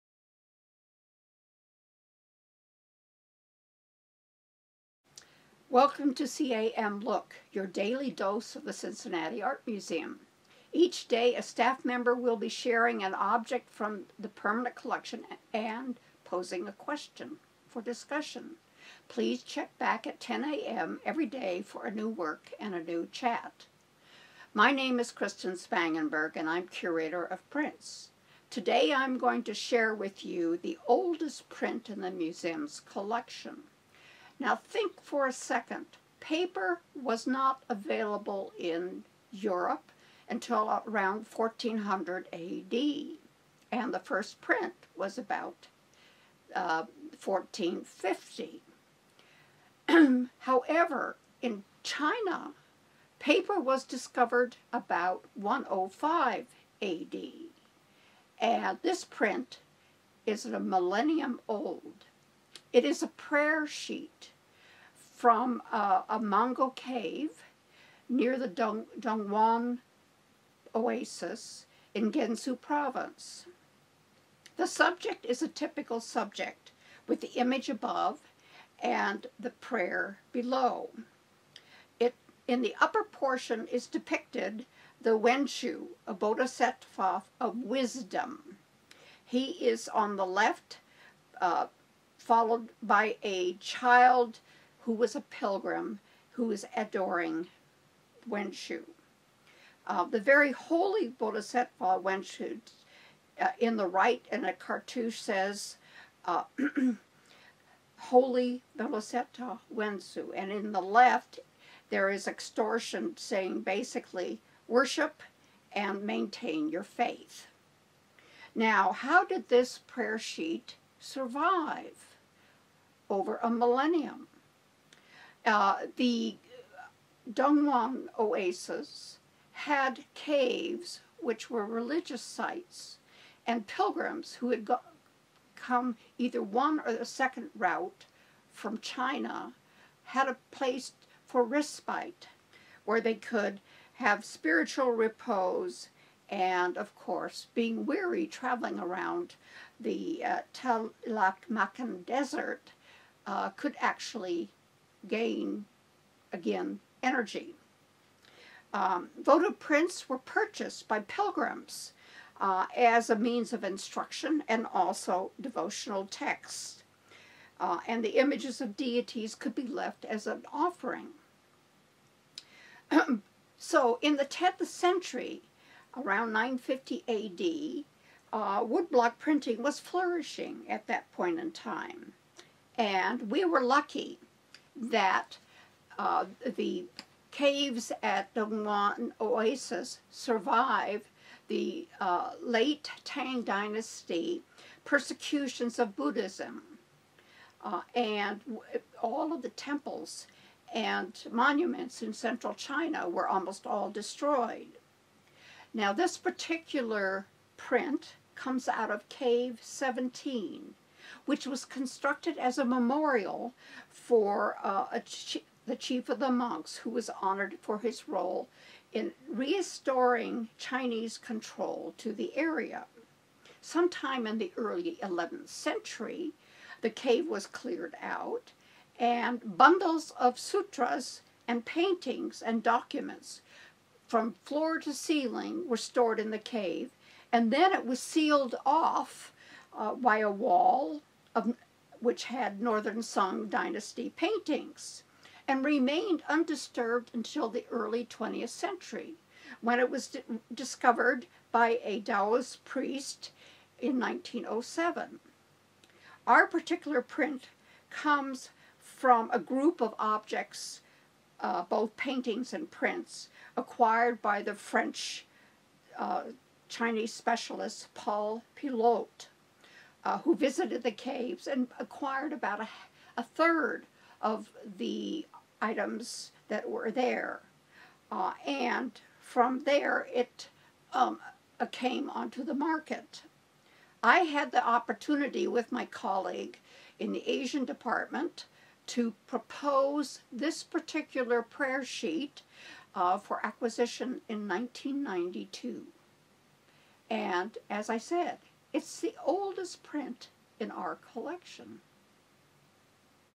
CAM Look | Prayer Sheet from Dunhuang, China | 5/26/21
Today, Kristin Spangenberg, our curator of prints, shares a Prayer Sheet from Dunhuang, China.

Originally posted on May 26, 2021, on the museum's CAM Connect Facebook Group

Please take a moment and share your thoughts on our virtual programming.

Unidentified Artist, Prayer-Sheet from Dunhuang, China, Circa 950 CE, woodcut, The Albert P. Strietmann Collection, 1992.139